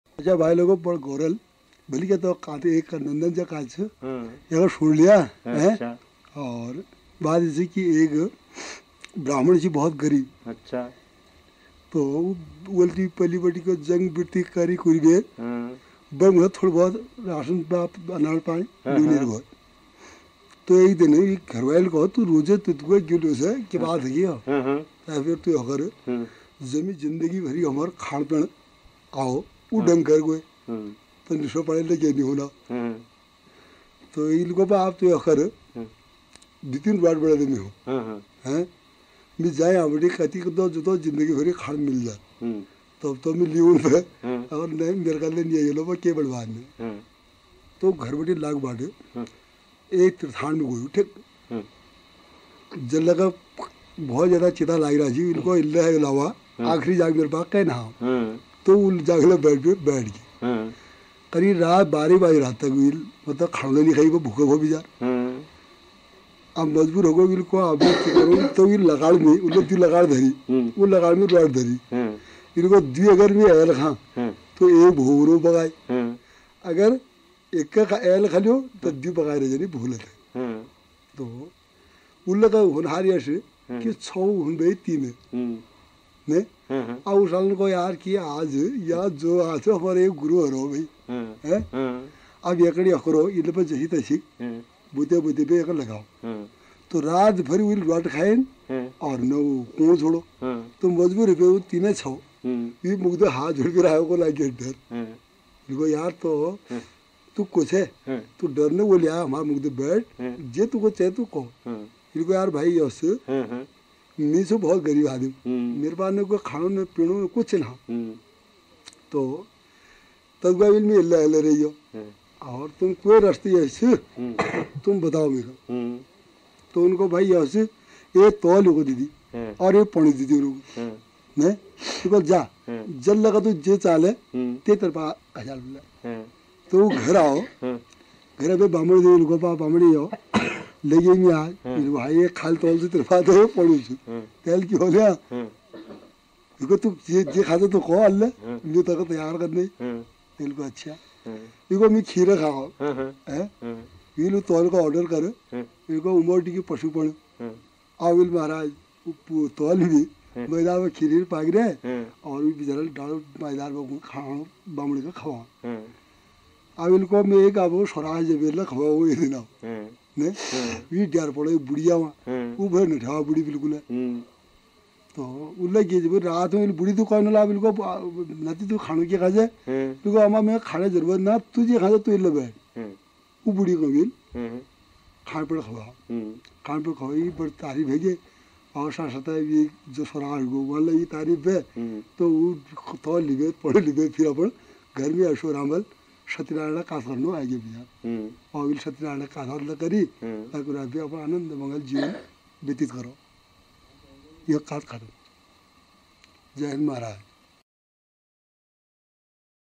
Lok Katha : Brahman Aur Bhoot | लोक कथा: ब्राह्मण और भूत - Mathura Datt Mishra | मथुरा दत्त मिश्रा
Lok Katha : Brahman Aur Bhoot |  लोक कथा: ब्राह्मण और भूत
Mathura Datt Mishra | मथुरा दत्त मिश्रा
Recording Year : 1970
Recording Place : Almora
Recorded By : Marc Gaborieau